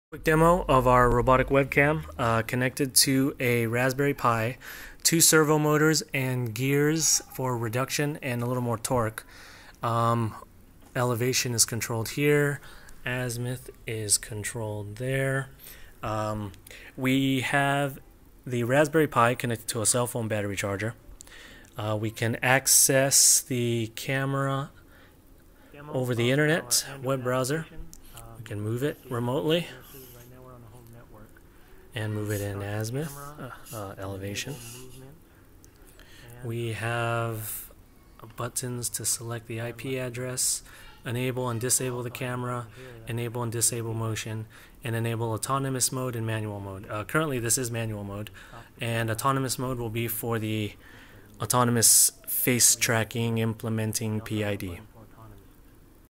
Cellphone/Remote Robotic Webcam Positioner CSUSB CSE 541 Robotics 85%
Quick demo of our project. Remotely control a robotic webcam from a custom Android application or through any web browser across the internet and WIFI.